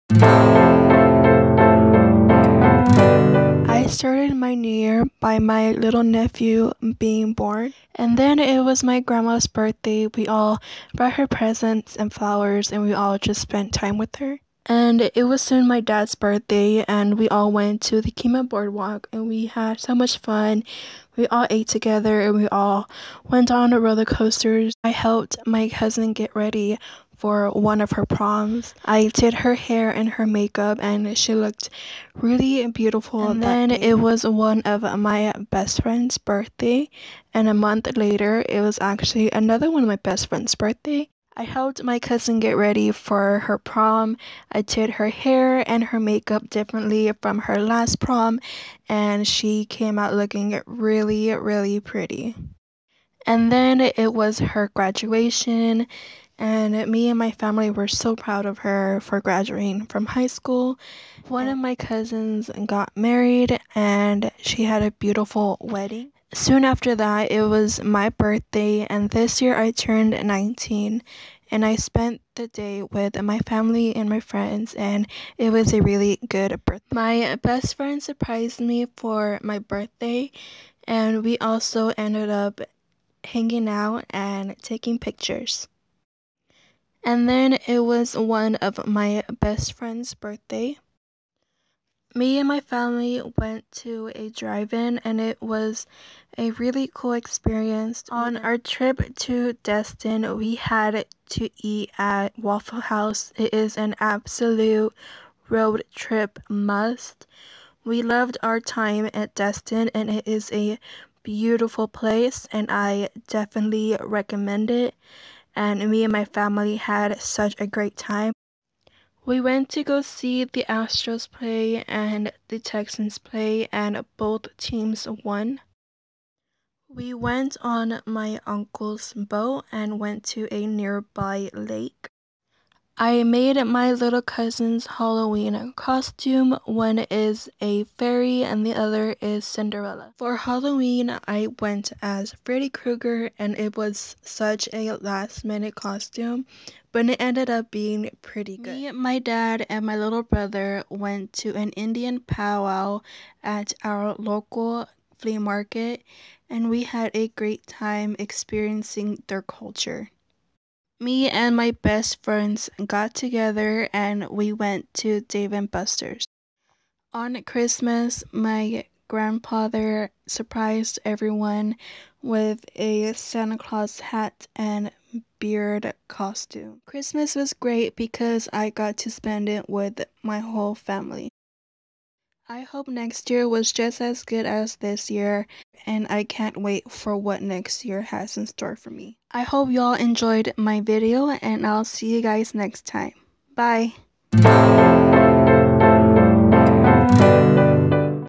2015 Recap | Adalina
Happy New Years to everyone! I made this video to share with y'all how 2015 was for me. This year has been an amazing year. I was a bit like a rollercoaster but at the end it was a great year. I hope y'all enjoy my video.